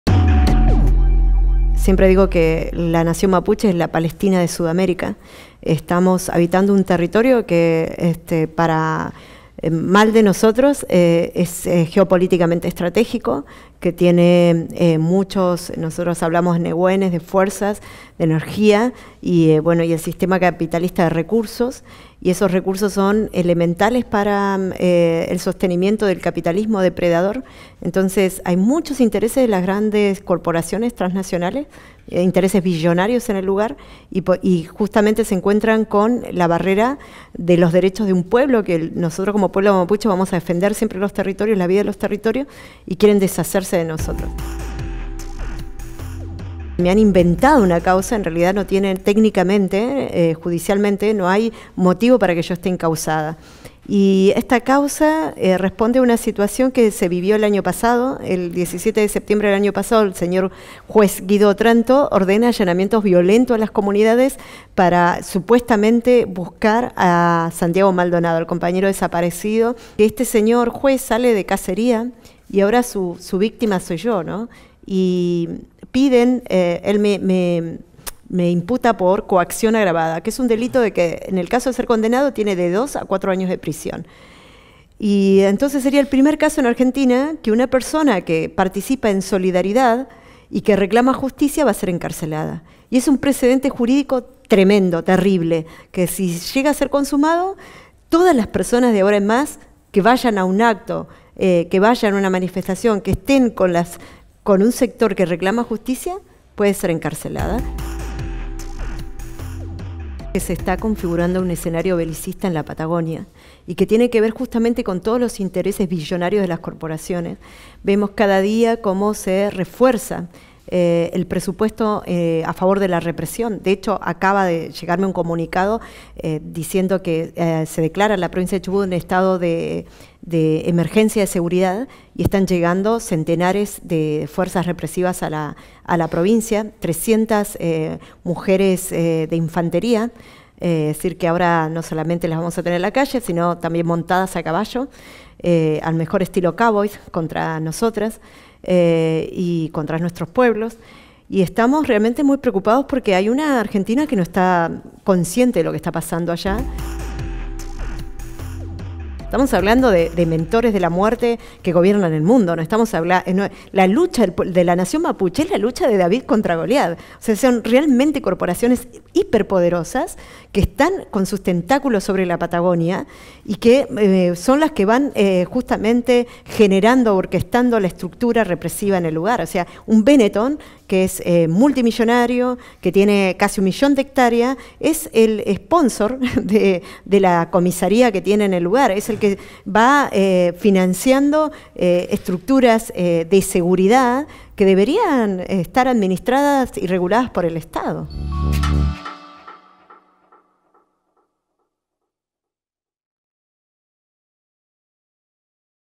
Moira Millán | “La nación mapuche es la Palestina de Sudamérica”
Entrevista a Moira Millán, referente mapuche procesada por defender territorios ancestrales por el juez que no investigó la desaparición de Santiago Maldonado.
En el marco de la persecución judicial a las comunidades mapuche del sur, el pasado 26 de junio Moira Millán fue notificada que por orden del Juez Guido Otranto que está imputada bajo la figura de  “Coacción Agravada” por el reclamo realizado el 20 de septiembre de 2017 en el Tribunal de Esquel. “Me han inventado una causa, técnicamente no tienen motivos para encausarme”, asegura en esta entrevista la weichafe mapuche.